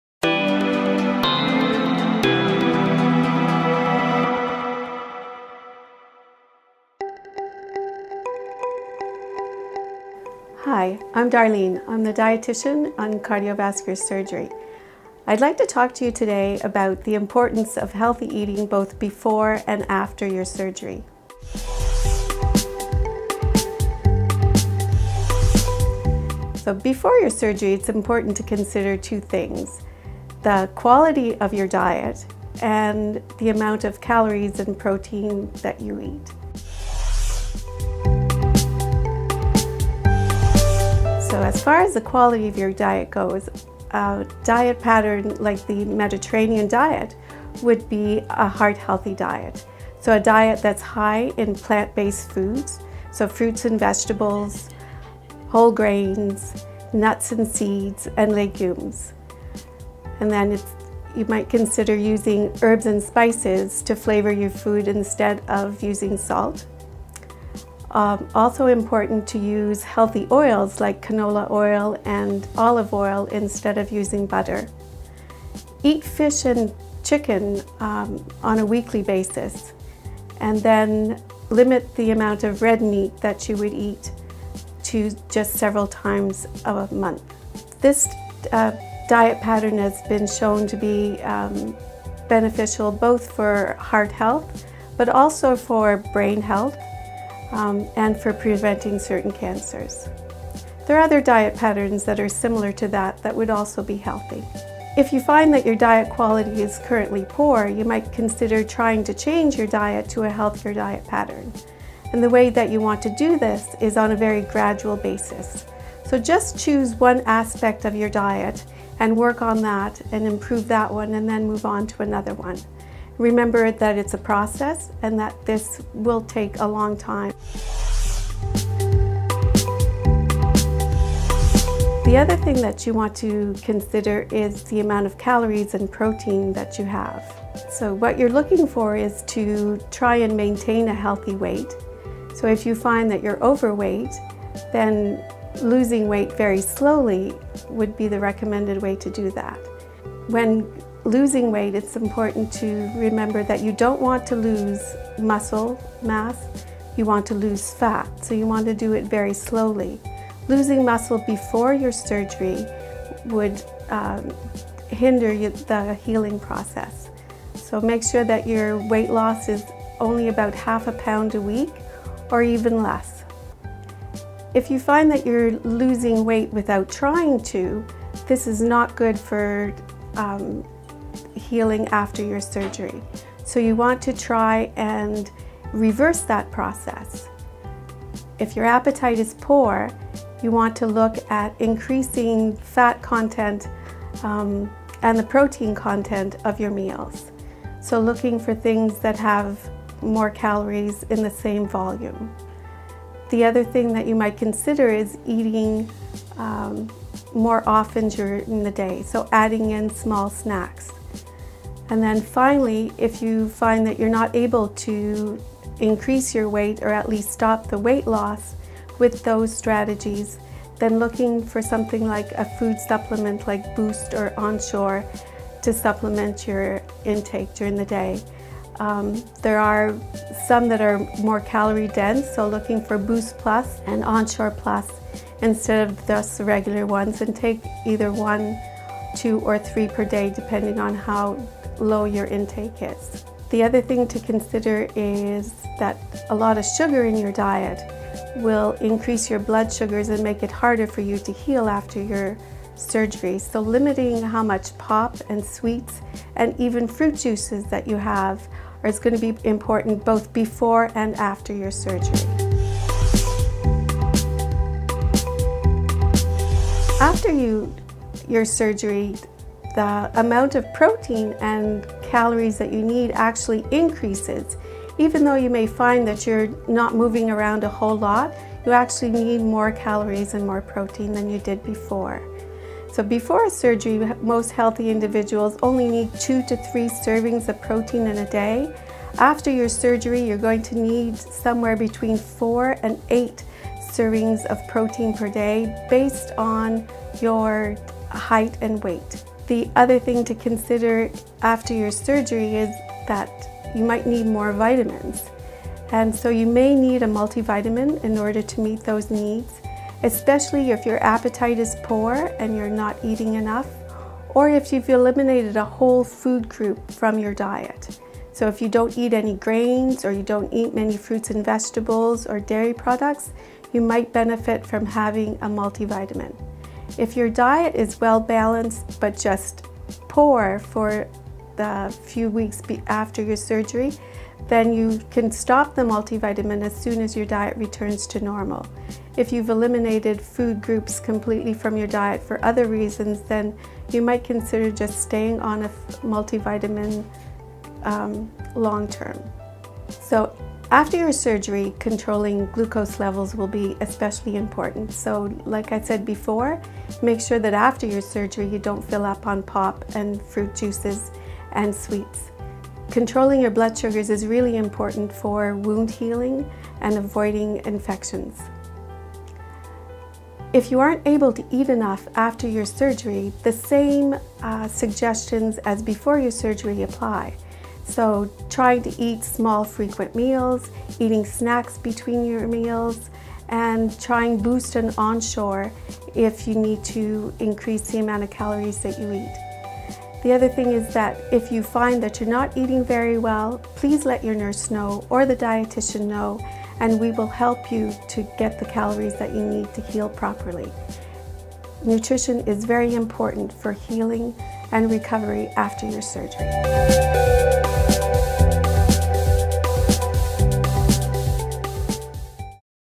Enhanced Recovery CARDIAC SURGERY
Enhanced recovery after surgery
Diet before and after Cardiac surgery